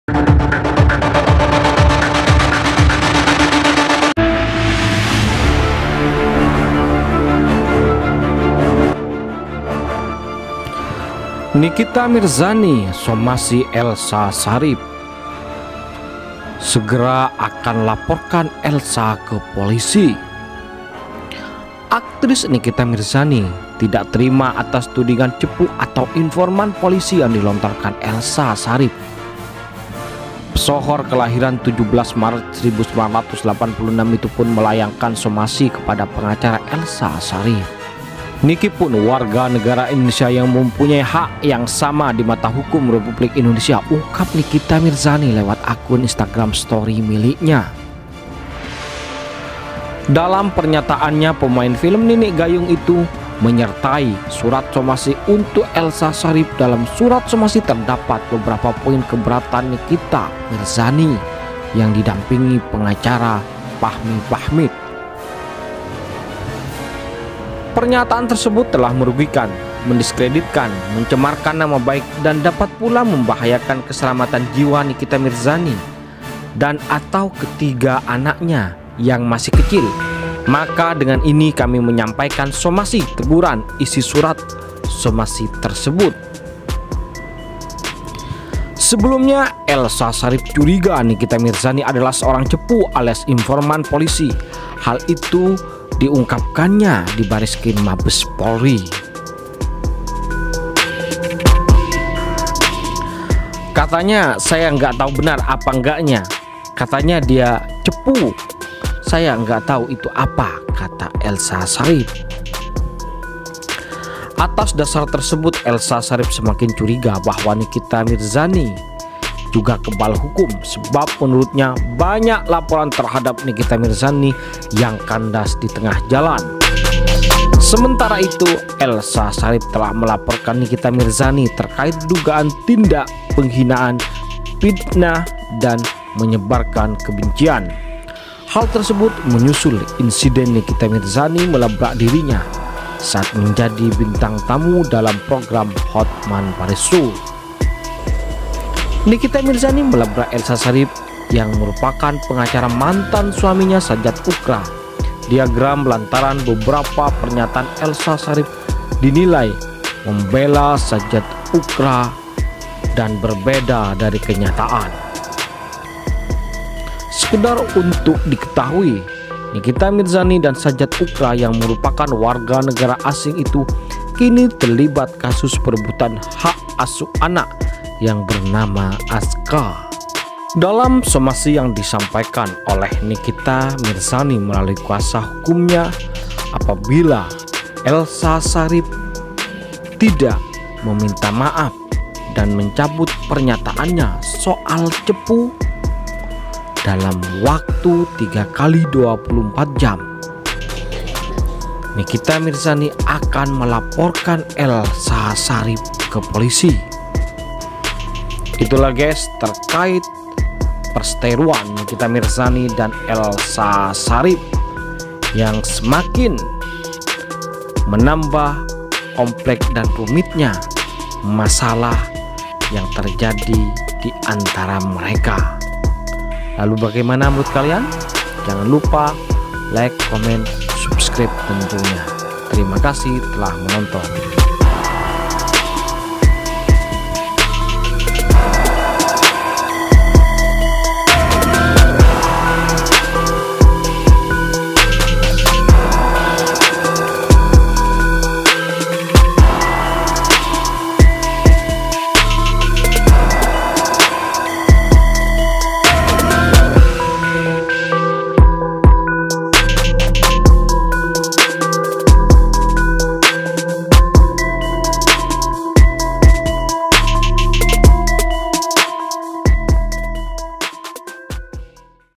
Balasan Nikita Mirzani, 50M45i Elza Syarief SGR L4p0rk4N Elza dlm K45U5 Lain, T0€ duh4n dan F¡t N4H?
Balasan Nikita Mirzani, 50M45i Elza Syarief Segera L4p0rk4N Elza Atas K45U5 Lain, T0€ duh4n dan F¡t N4H??

Jangan Lupa: _
Thankz!!..